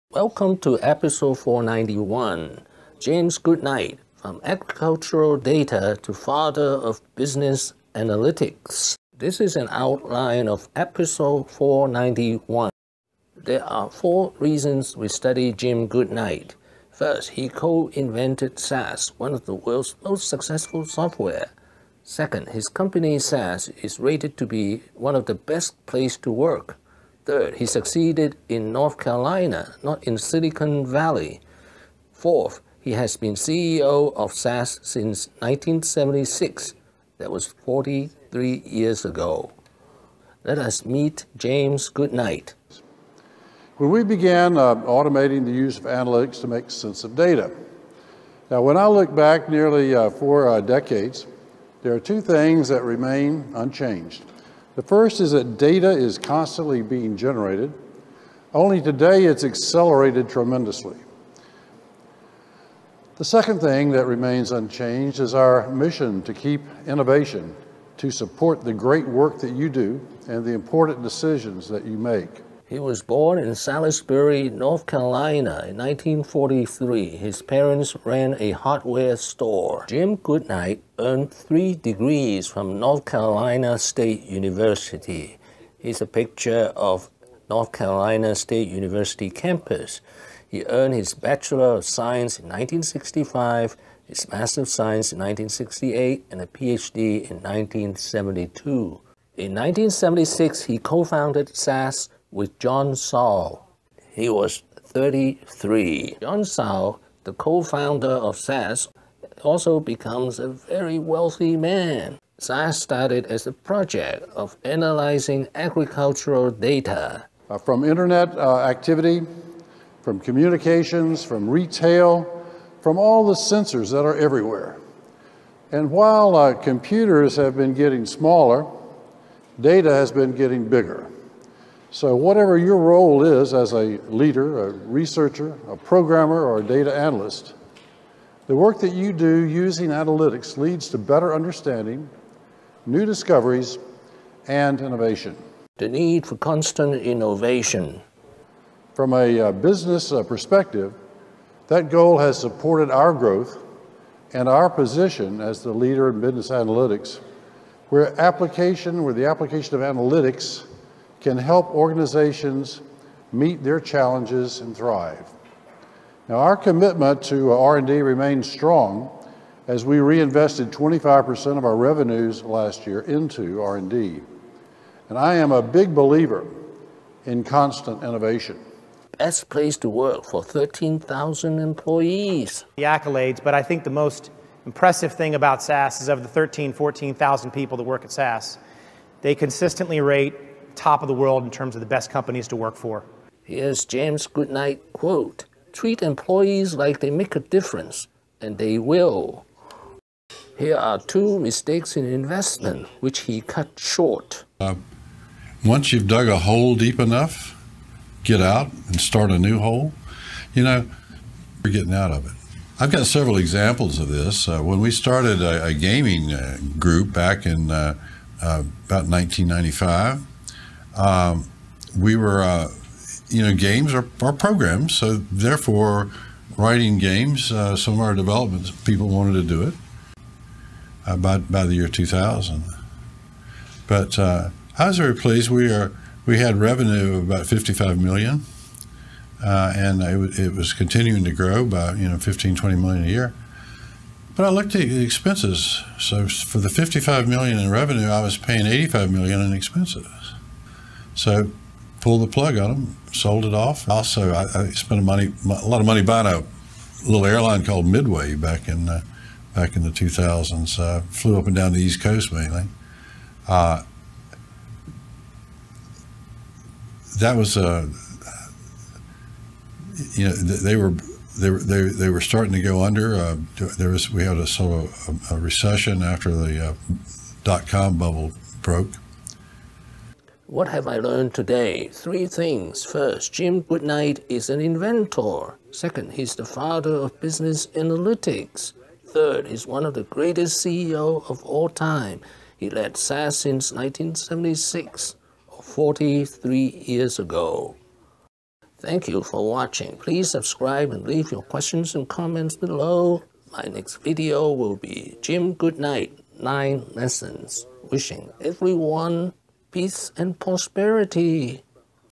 SecretsSelfmadeBillionaires0491 James Goodnight Father ofSAS &Business Analytics frmNorthCarolina
James Goodnight co-invented SAS with John Sall in 1976 after earning PhD in North Carolina State University.  He is father of Business Analytics or Statistical Software and has been CEO since 1976, a feat of 43 years. SAS has been consistently voted one of best place to work in world.  Employee benefits are important. He made some mistakes too but he cut short his losses. SAS reinvented 25% of revenue into R&D and constant innovation is his secret of success